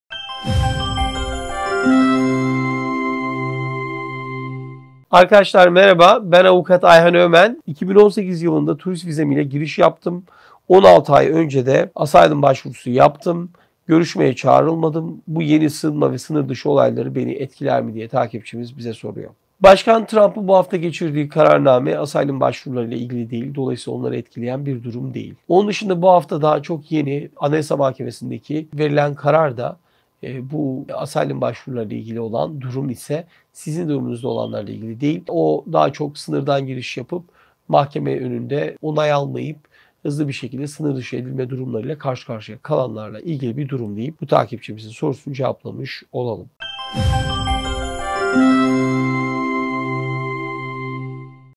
AMERİKA’YA İLTİCA EDENLER BAŞKAN TRUMP’IN KARARNAMESİNDEN ETKİLENECEK Mİ ?
Soru: 2018 yılında turist vizem ile giriş yaptım. 16 ay önce de aslyum başvurusu yaptım, görüşmeye çağrılmadım. Bu yeni sığınma ve sınır dışı olayları beni etkiler mi ?